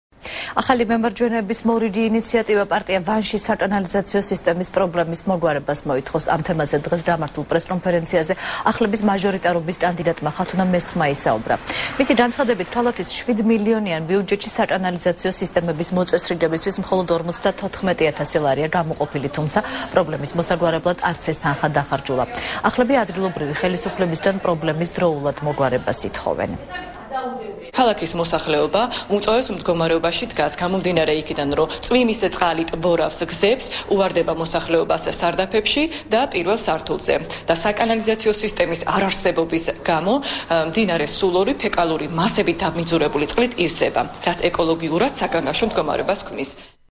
ახალი 6 | მემარჯვენეები ვანში | 25.08.12
"ახალი მემარჯვენეების" მორიგი ინიციატივა - პარტია ვანში საკანალიზაციო სისტემის პრობლემის მოგვარებას მოითხოვს. ამ თემზე დღეს გამართულ პრესკონფერენციაზე „ახლების" მაჟორიტარობის კანდიდატმა  ხათუნა მესხმა ისაუბრა. მისი განცხადებით , ქალაქის 7 მილიონიან ბიუჯეტში, საკანალიზაციო სისტემების მოწესრიგებისთვის მხოლოდ 54 ათასი ლარია გამოყოფილი, თუმცა, პრობლემის მოსაგვარებლად არც ეს თანხა დახარჯულა.  „ახლები" ადგილობრივი ხელისუფლებისგან პრობლემის დროულად მოგვარებას მოითხოვენ .